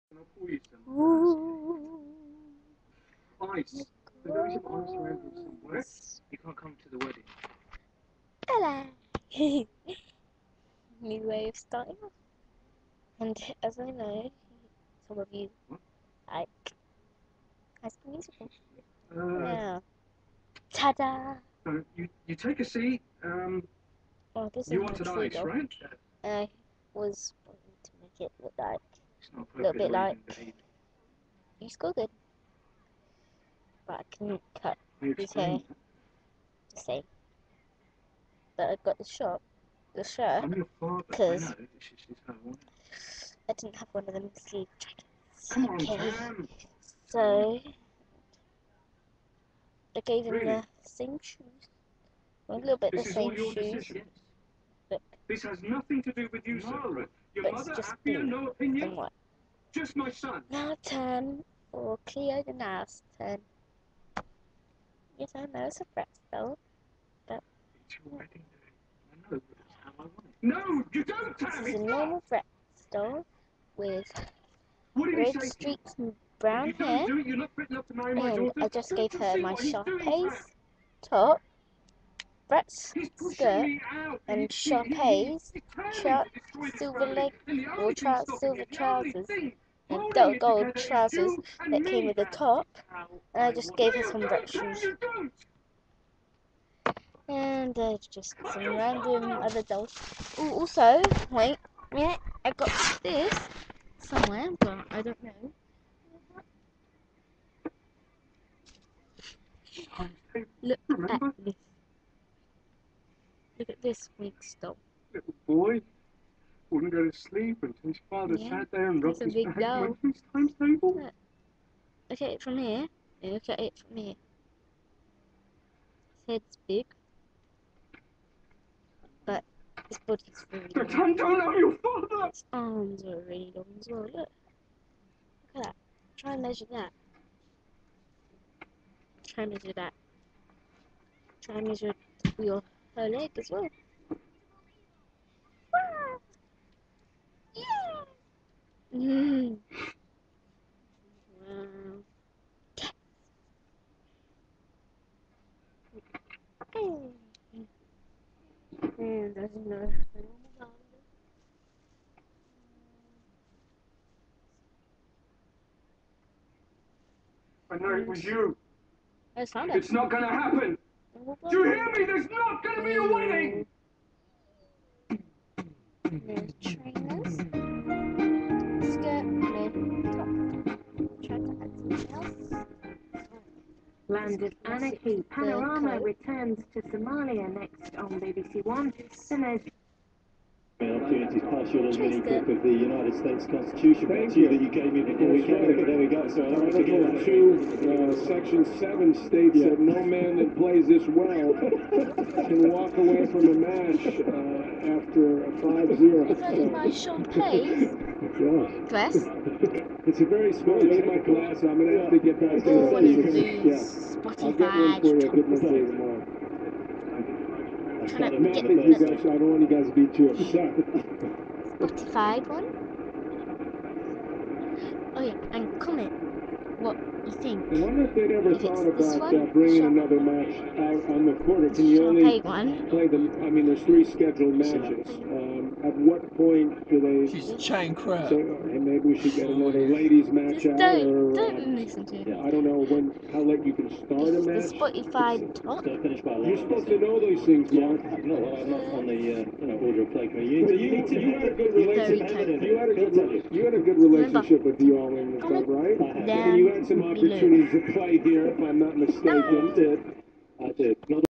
2 Monster high look-a-likes, 1 winx and 1 barbie.
2 monster high look-a-likes!
Comment on what i should put on my barbie doll!